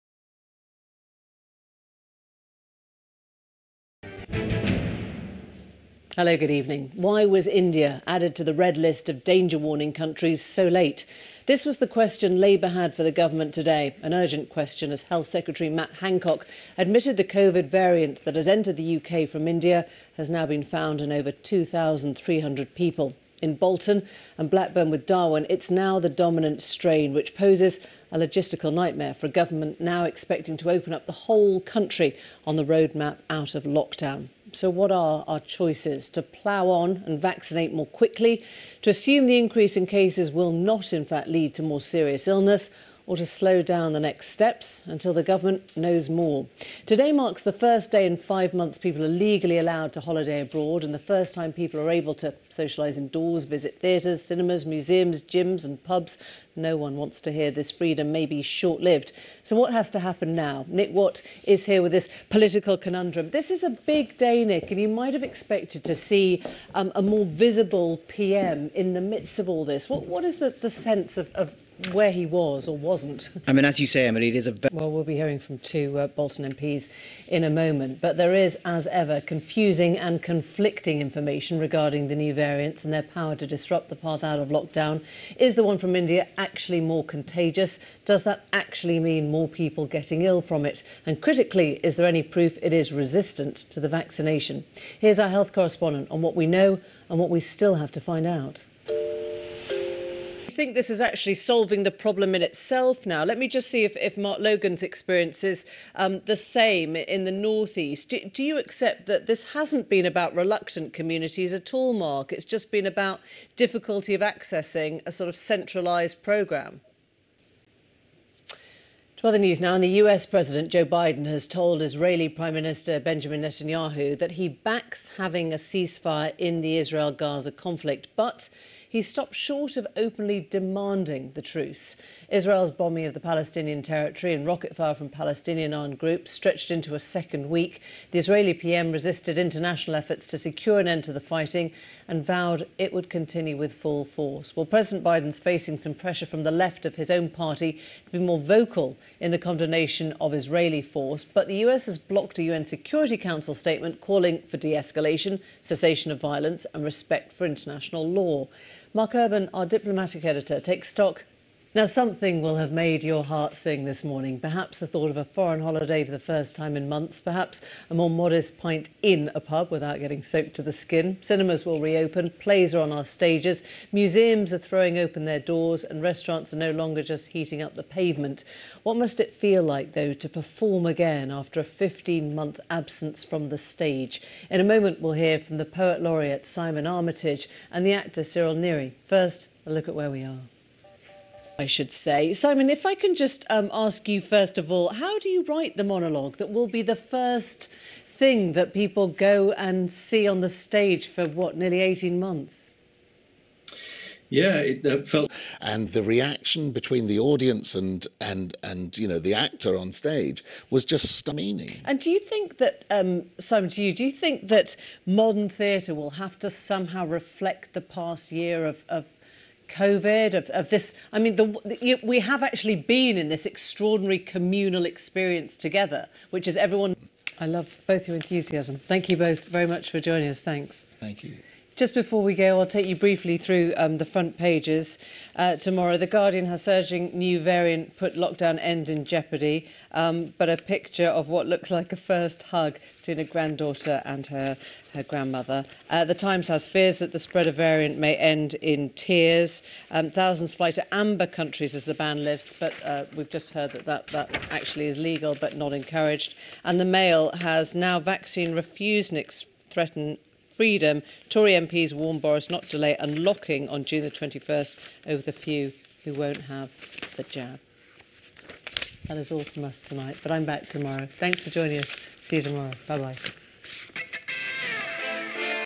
Emily Maitlis - Black Tights - 17/5/21
Gorgeous news presenter Emily Maitlis in black tights & boots!

17/5/21